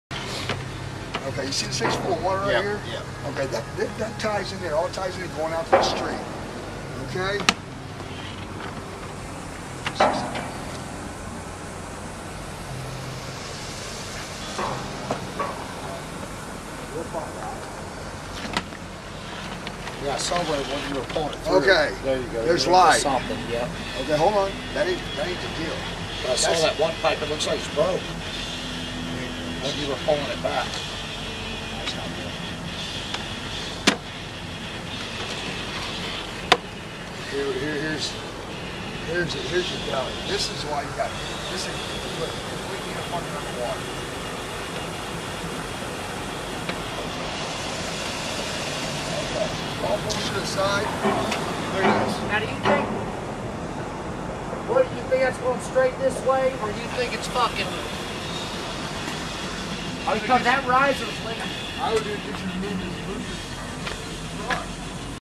Plumbing-Camera Drain Pipe-HoustonTx-TheZuell.MP4
This is a drain pipe we ran a camera line down in it and discovered it had a low spot and was holding water.